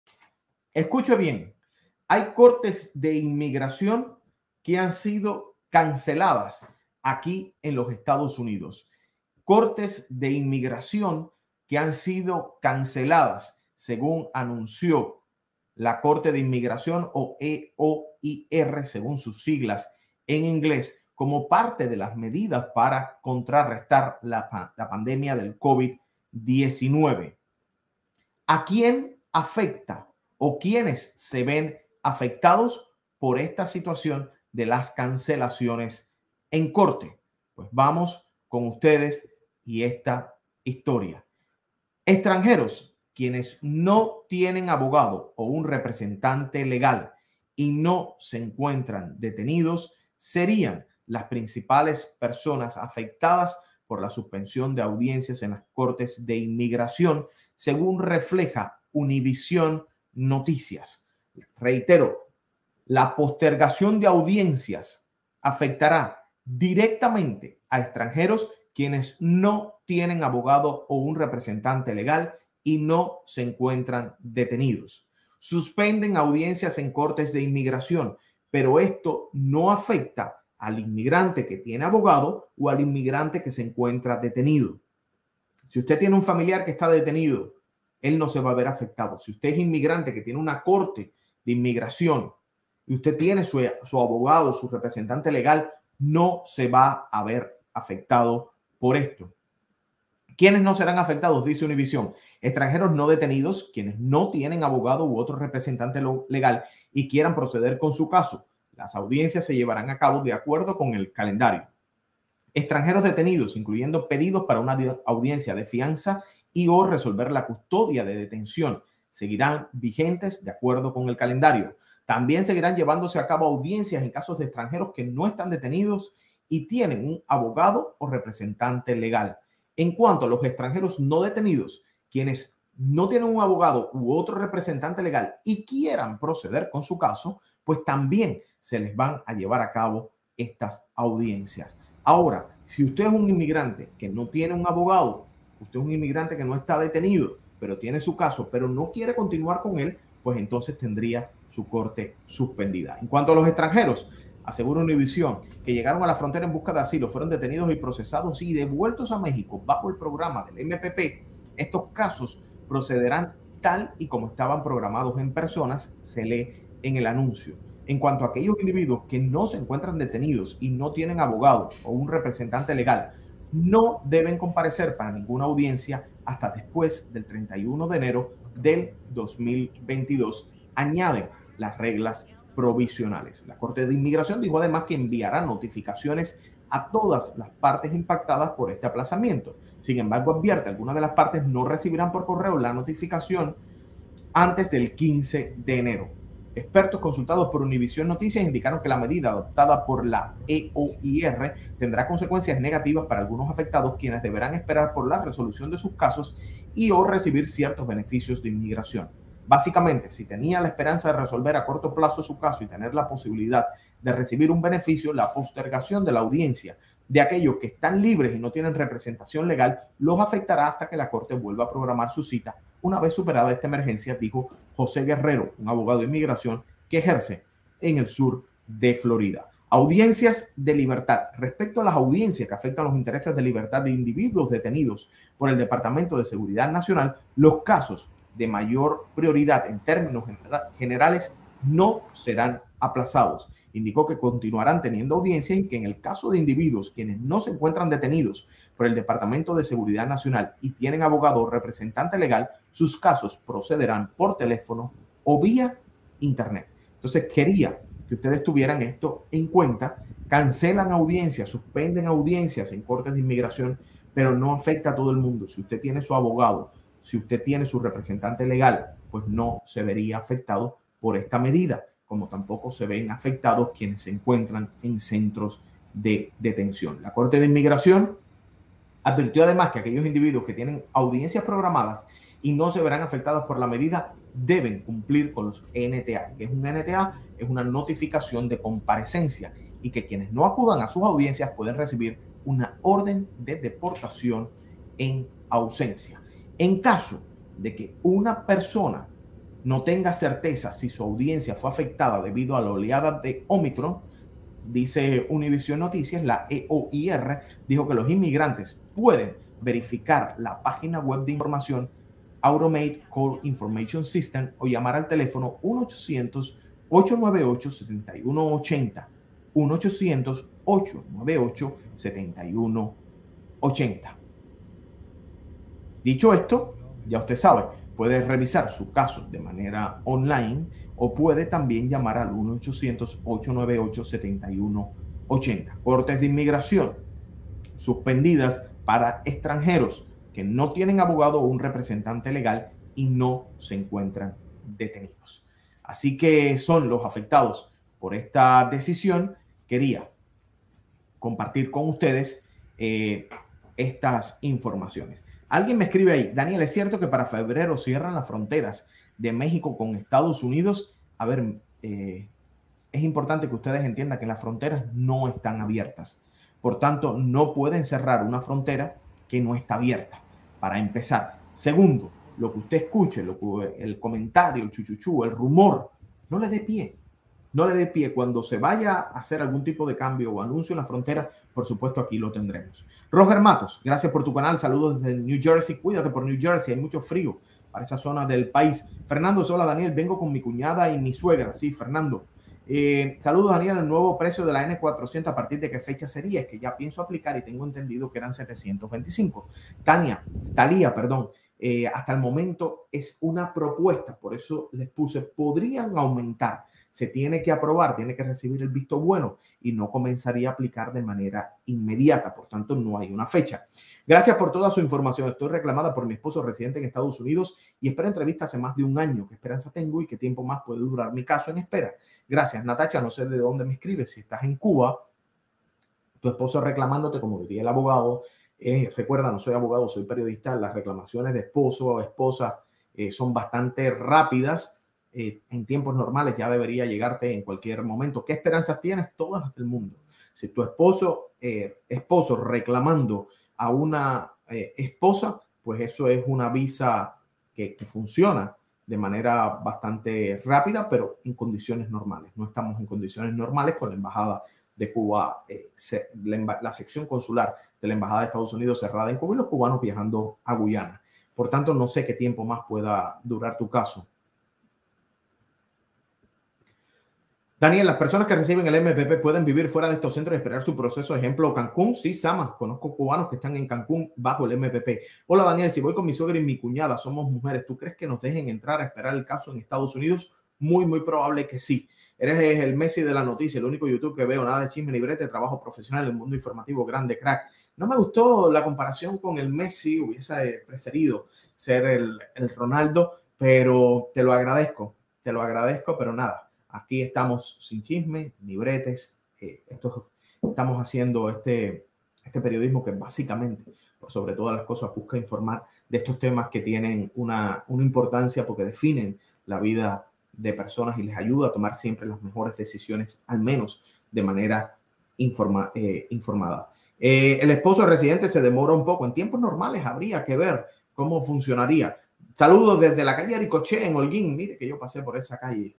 CONOZCA QUÉ MIGRANTES SE VEN AFECTADOS POR CANCELACIONES DE CORTES DE INMIGRACIÓN EN ESTADOS UNIDOS
CONOZCA QUE MIGRANTES SE VEN AFECTADOS POR CANCELACIONES DE CORTES DE INMIGRACIÓN EN LOS ESTADOS UNIDOS Y EL IMPACTO QUE TIENE EN SUS CASOS.